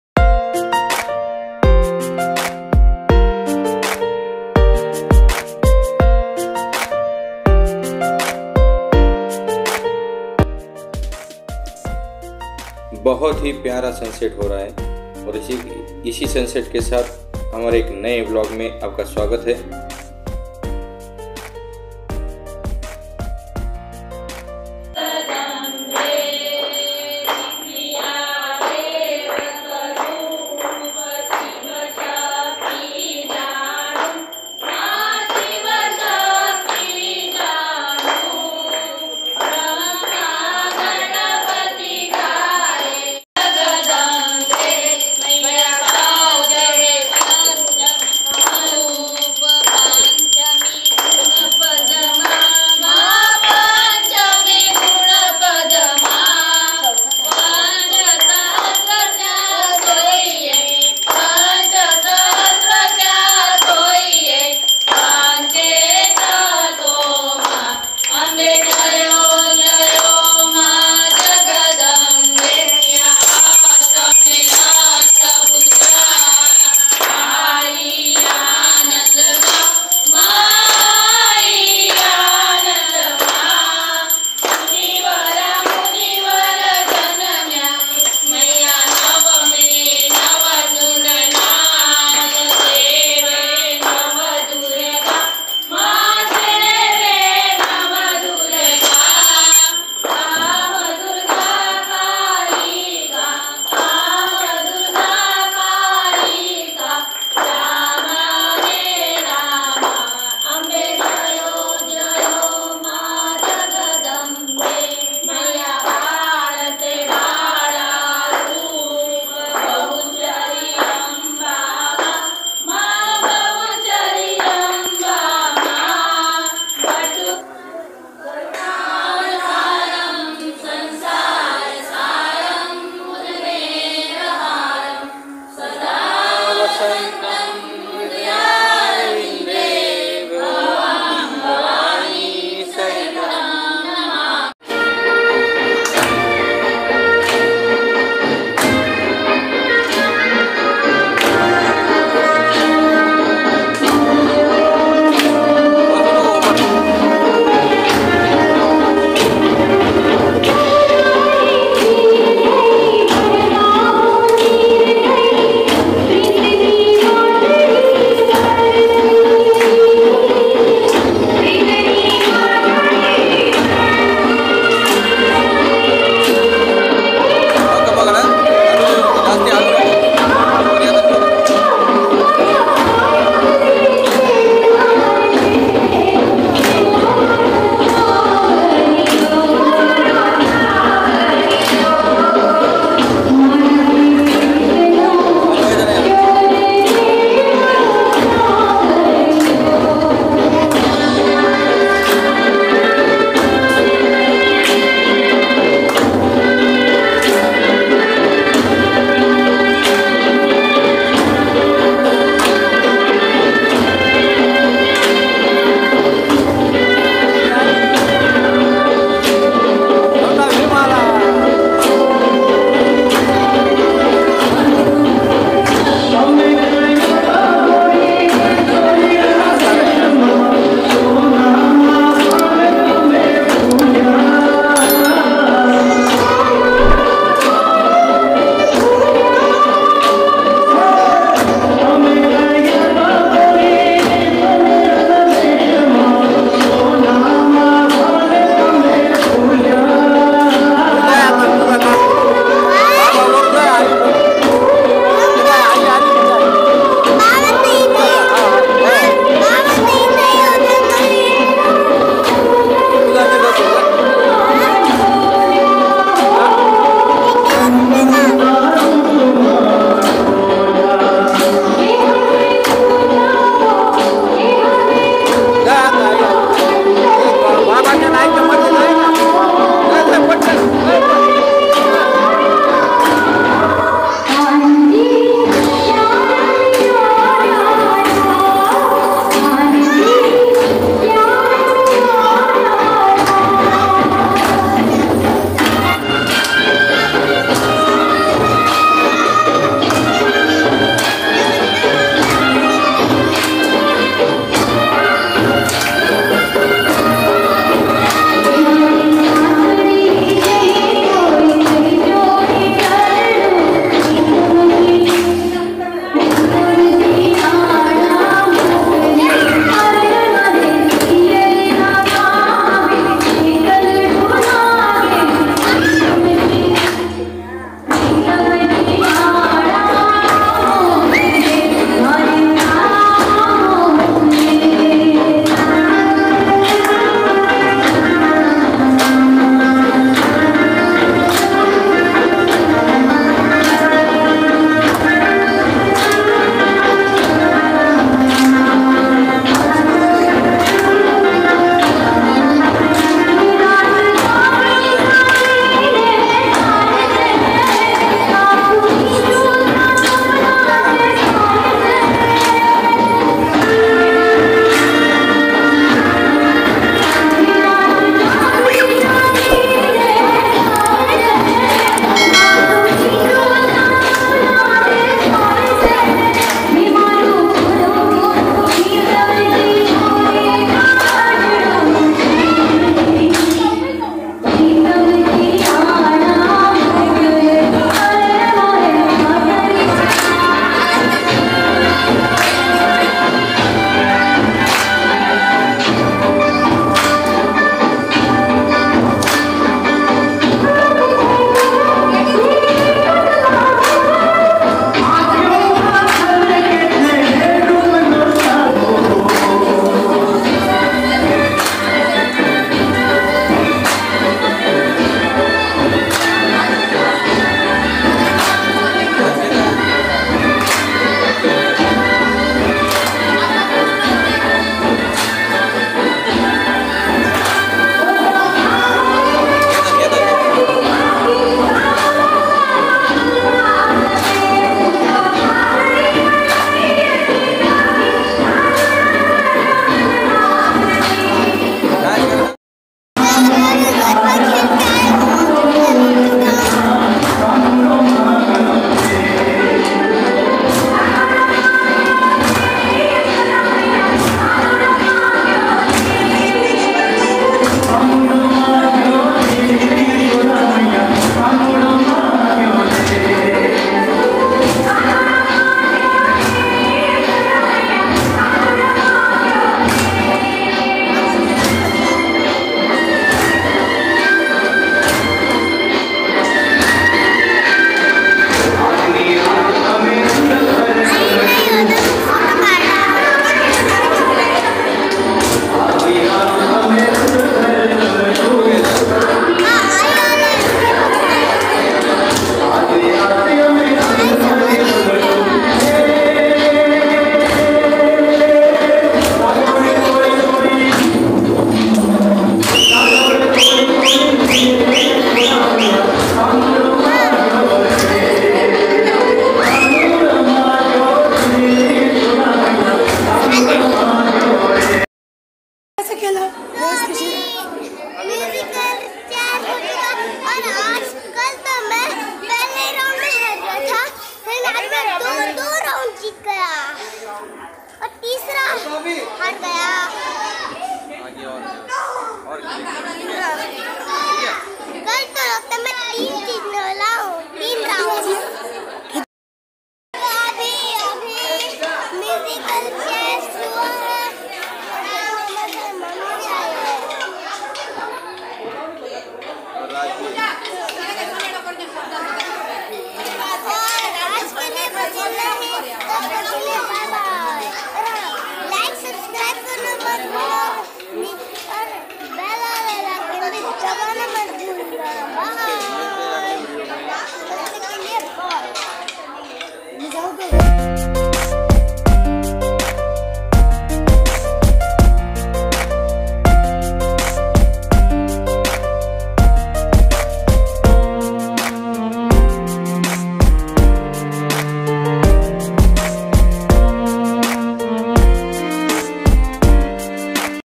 Navratri Aarati,Garba & Dandiya |Day 4 | Anvit is learning Garba dance | Vapi | Gujarat | Vlog
Today is day 4 of Navratri Garba and dandiya for the year 2022. Anvit is learning Garba dance also I tried too.
Channel Name - Shree Mahajan Vlogs